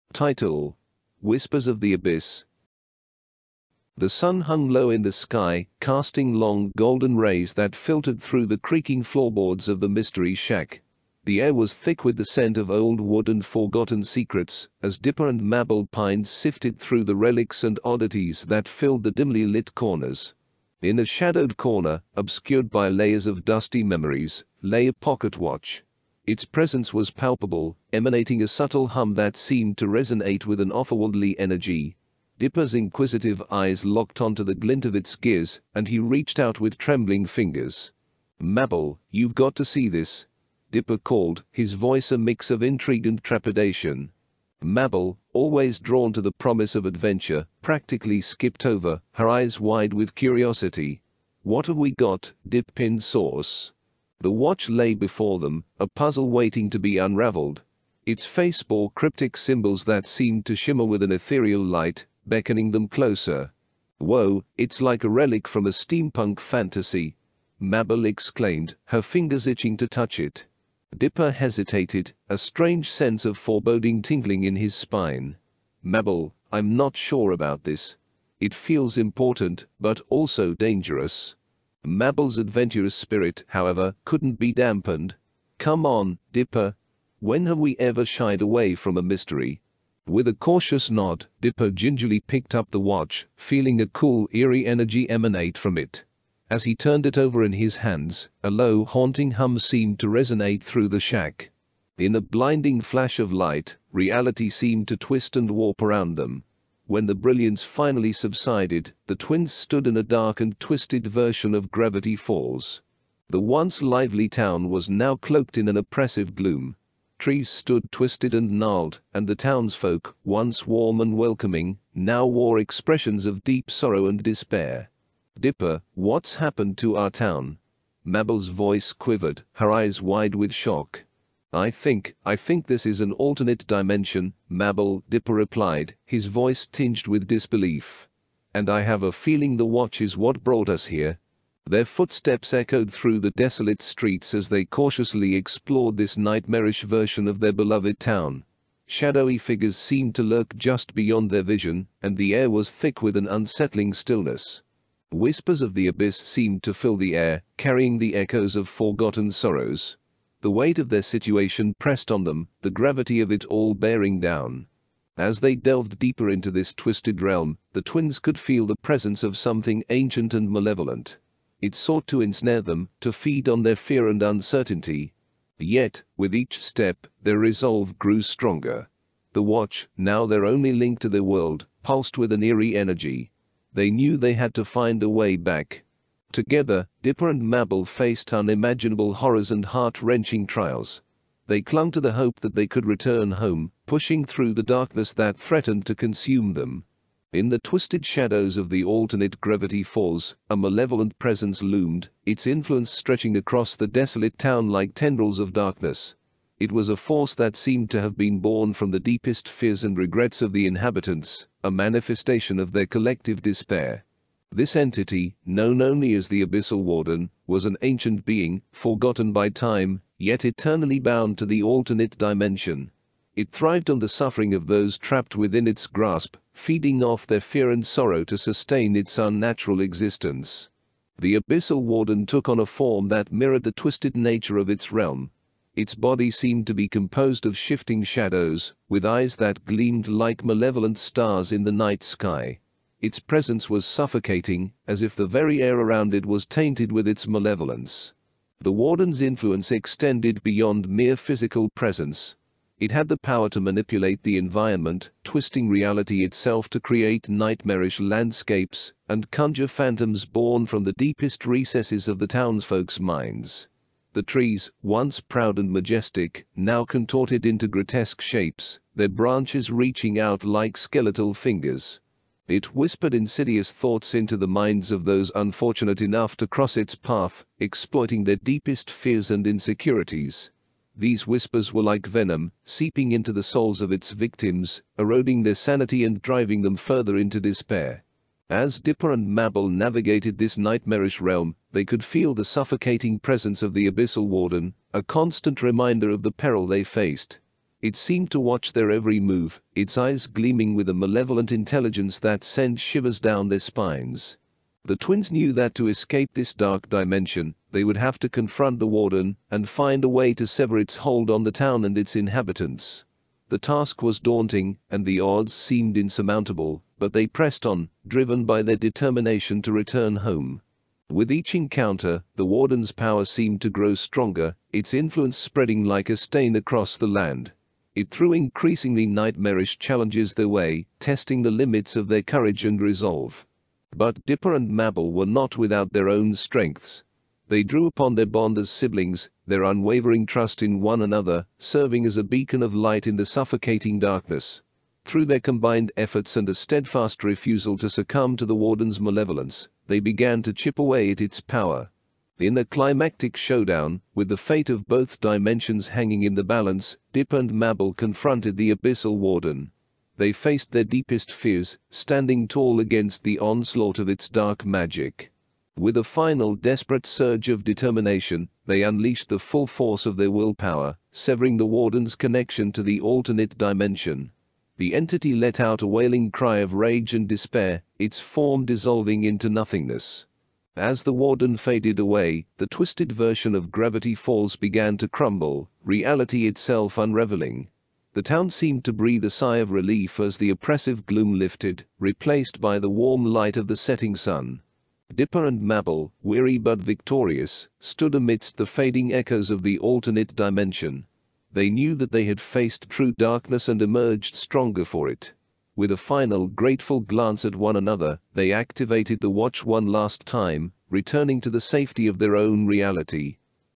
ChatGPT Audiobook: Gravity Falls Story "Whispers of the Abyss"
Another Gravity Falls themed ChatGPT story. Cool to see it write stories like this.